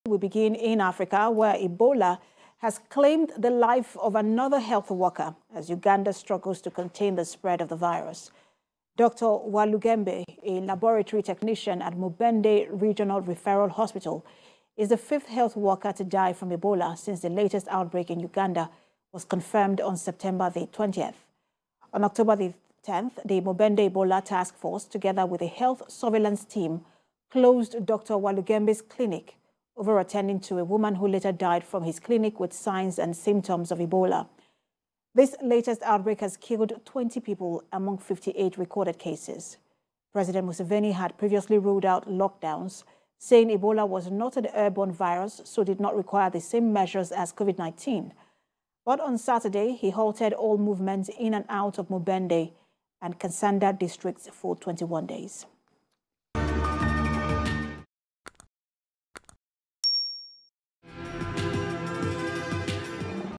UPDATE: Fifth Health Worker Dies of Ebola Virus in Uganda
Ebola virus has claimed the life of another health worker as Uganda struggles to contain the spread of the virus.

Dr. Walugembe, a laboratory technician at Mubende Regional Referral Hospital, is the fifth health worker to die from Ebola since the latest outbreak in Uganda was confirmed on September 20.